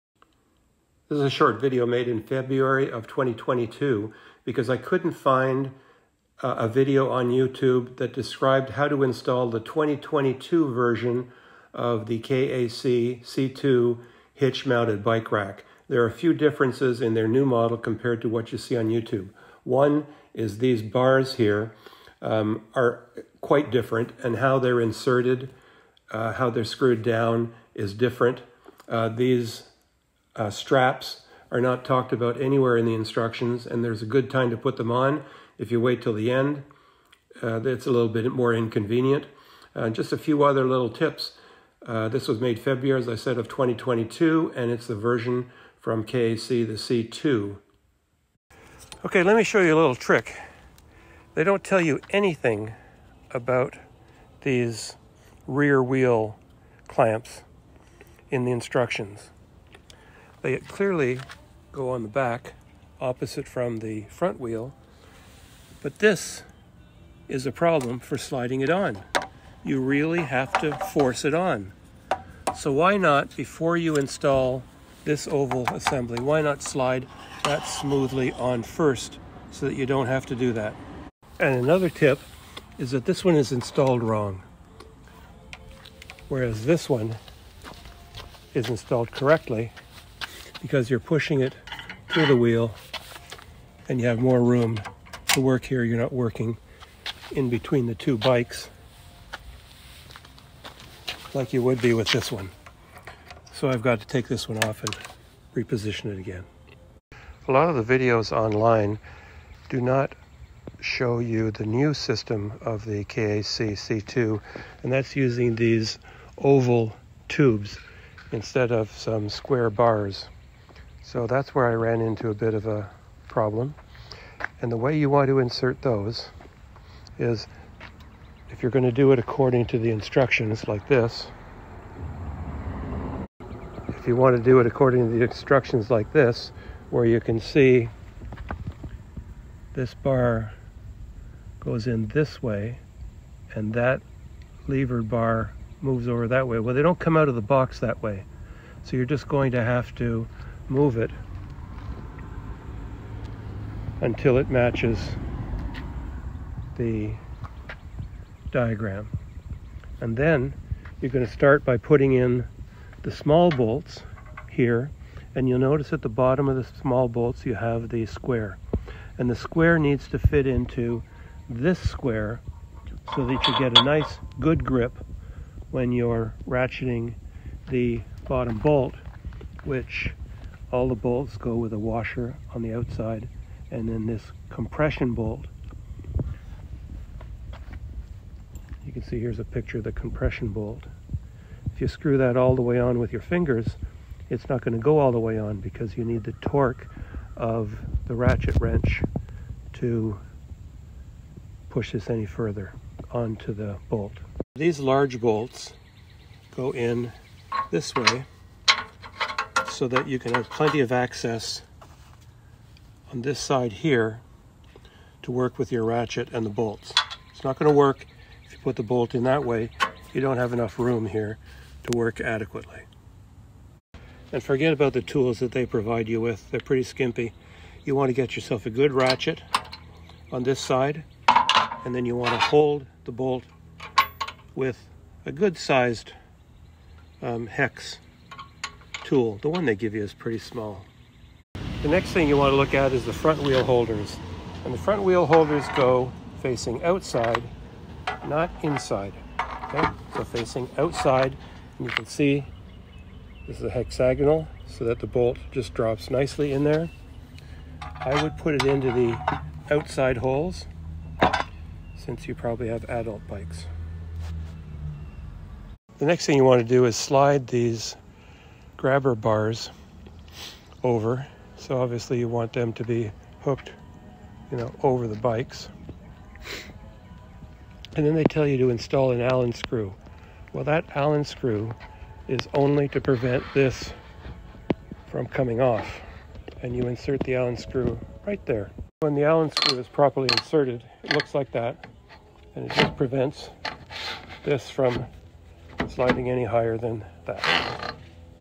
KAC C2 Bike Rack 2022 install tips
The 2022 version of this bicycle rack has changes that are noteworthy and I found that other YouTube videos were dealing with an older version￼